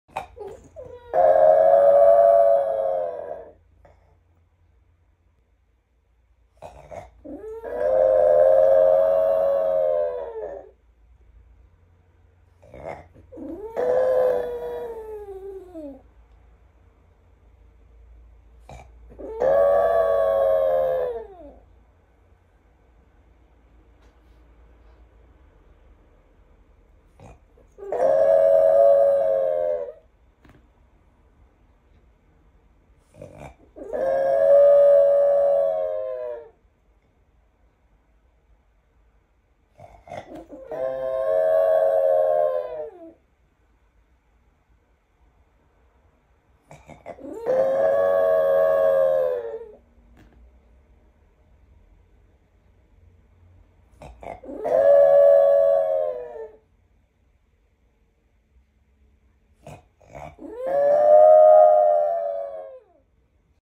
Chiyo crying after being left alone by Ash, our Shitzu, to go to the groomer.
The first time we heard my Chiyo the Frenchie cry… rather howl like a wolf 😂😂😂